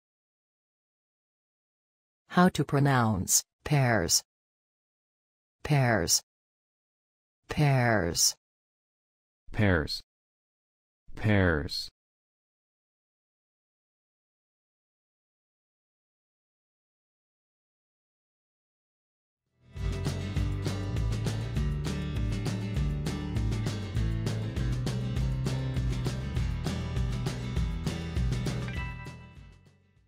Pairs pronunciation
How to pronounce Pairs in English? Listen with us.
What is the correct pronunciation of the word Pairs in everyday English? Speak as the Americans.
How to write the word Pairs? Watch and learn.

Learn how to pronounce Pairs.